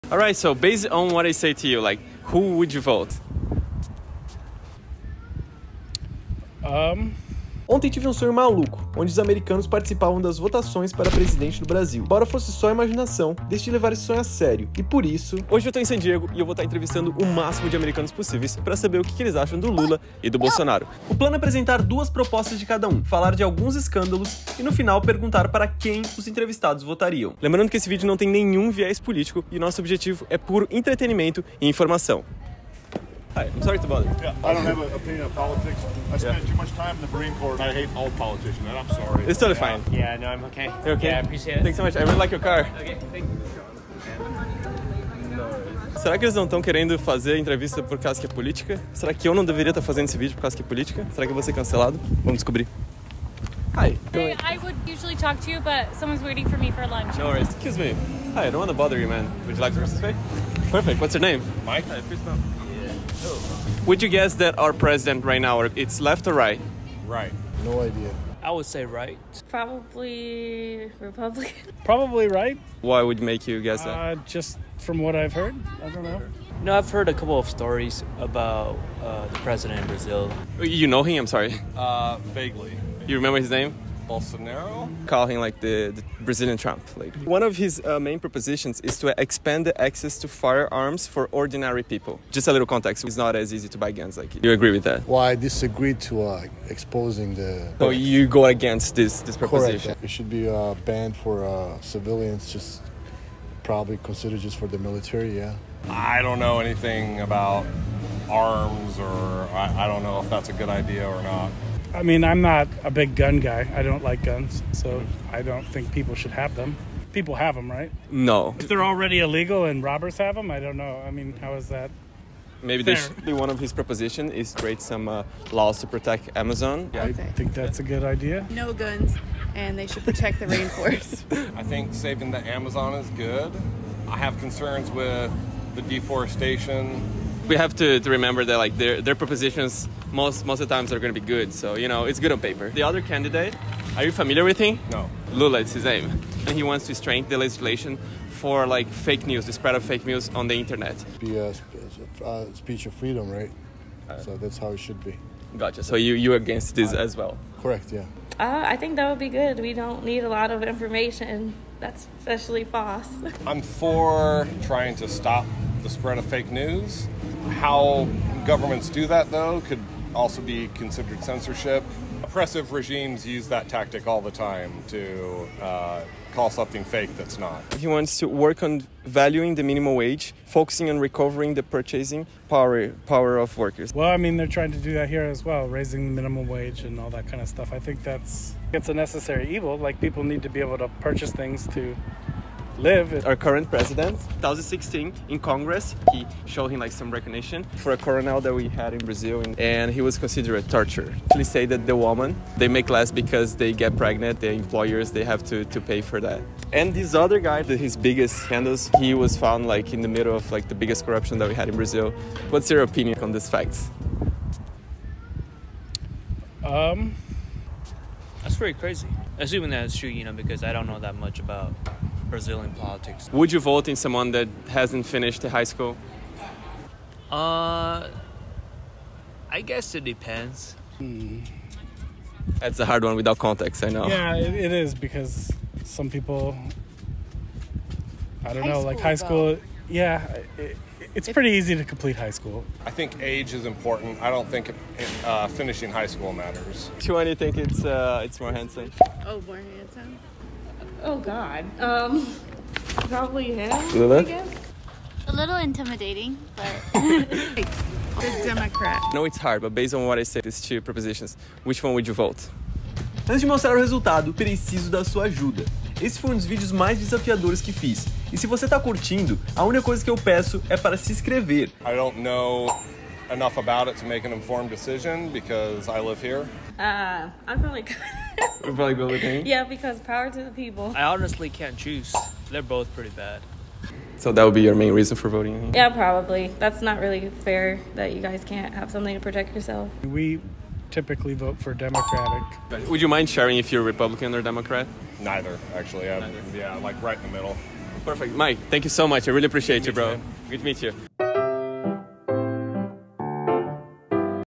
Em Quem Os Americanos Votariam? (Lula x Bolsonaro)
Dessa vez a missão foi descobrir em quem os americanos votariam para presidente do Brasil durante as eleições que estão prestes a acontecer.

Fica meu agradecimento a todos que aceitaram trocar uma idéia comigo!!